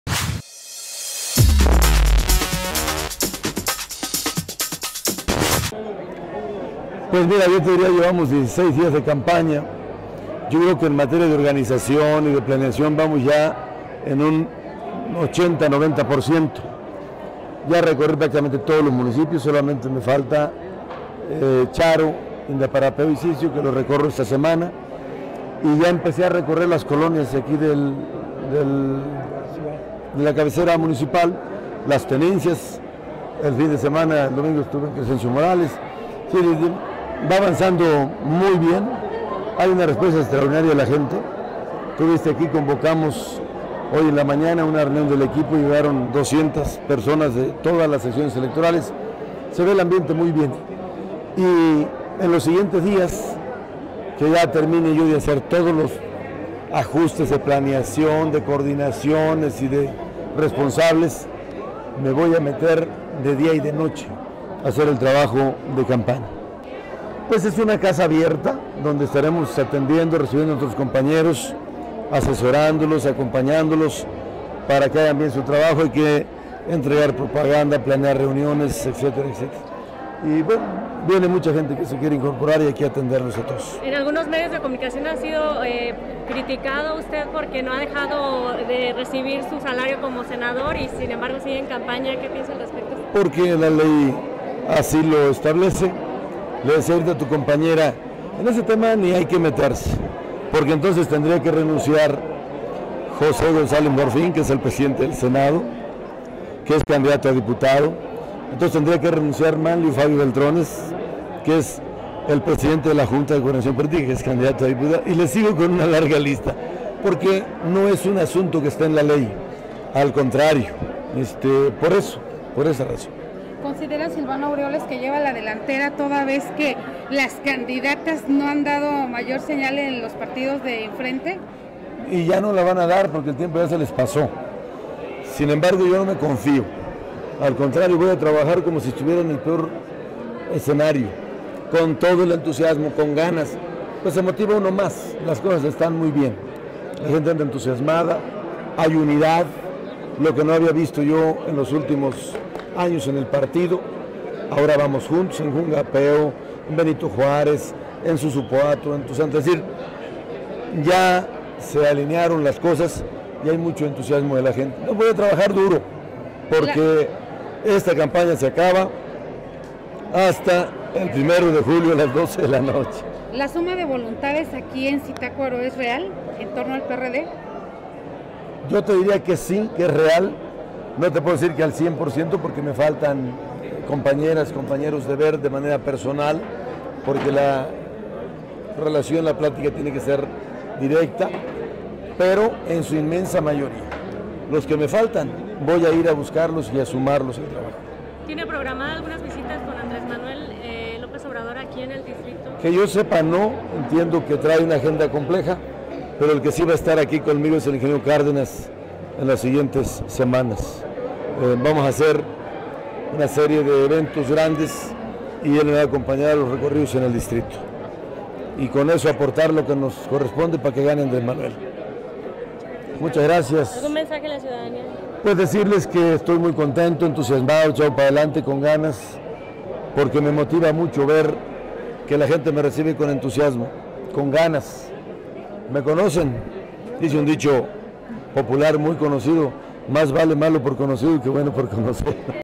Hay unidad en el partido: Silvano Aureoles - Zitácuaro - mizitacuaro.com
El candidato del PRD a la diputación federal por el distrito 03 con Sede en Zitácuaro, Silvano Aureoles Conejo, Aseguró  que a 16 días de campaña, en materia de organización y de planeación  se encuentran en un 80% - 90% completado, con excepción de tres municipios, ha recorrido casi todo el distrito y empezará a recorrer  las tenencias de la cabecera municipal. Silvano  expresó que a diferencia de otros años, "Hay unidad en el partido... se han alineado las cosas y  existe un gran entusiasmo en las personas".